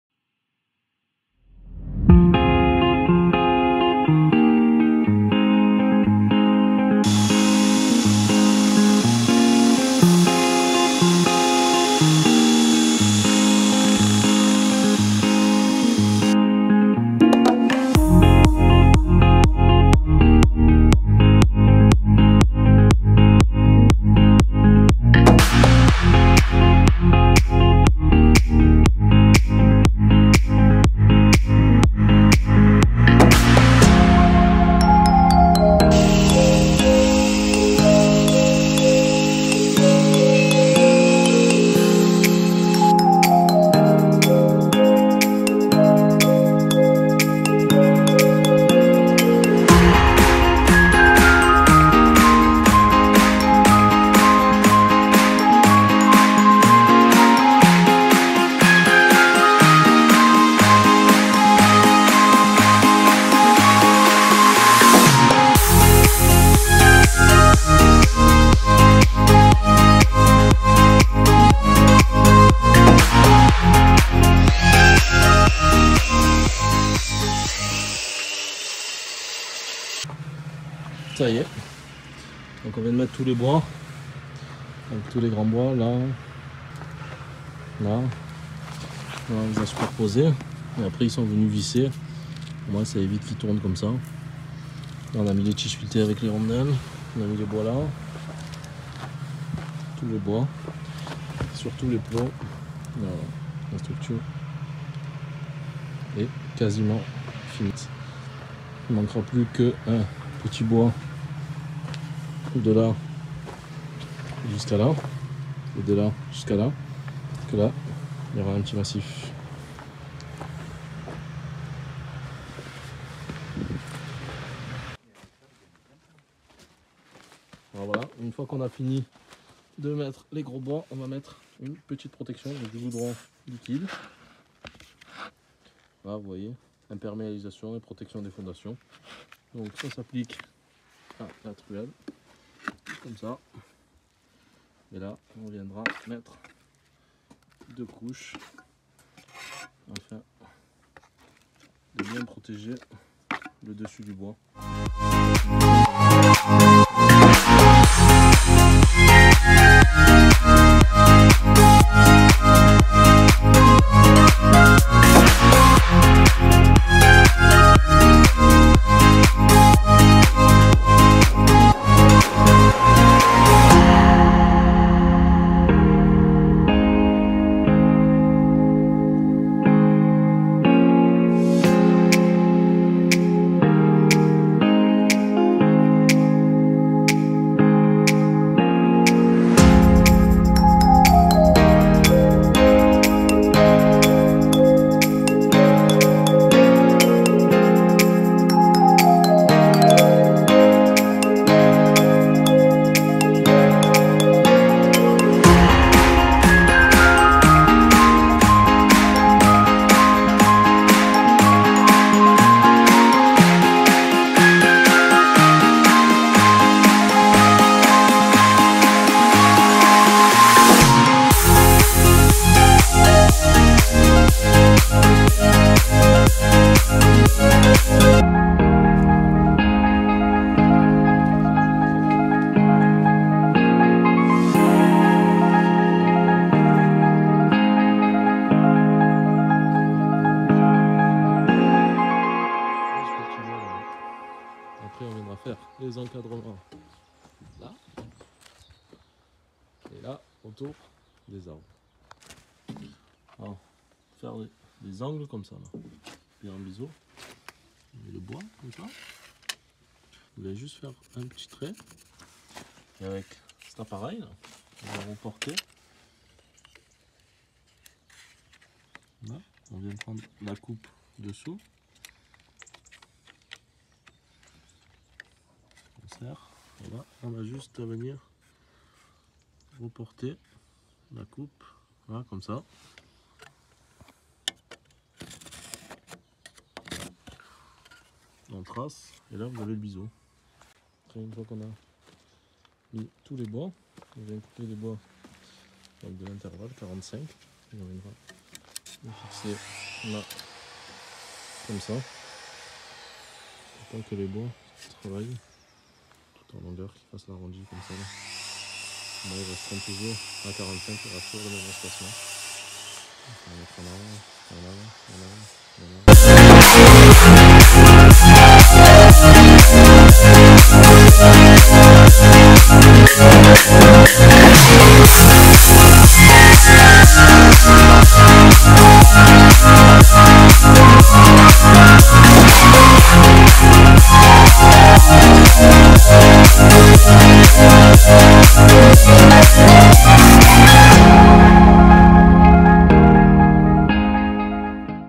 Je monte la structure en bois !! terrasse bois construire sa terrasse en bois
Nouvelle video
Je monte la structure bois qui viendras accueillir la terrasse en bois !

Musique proposée par La Musique Libre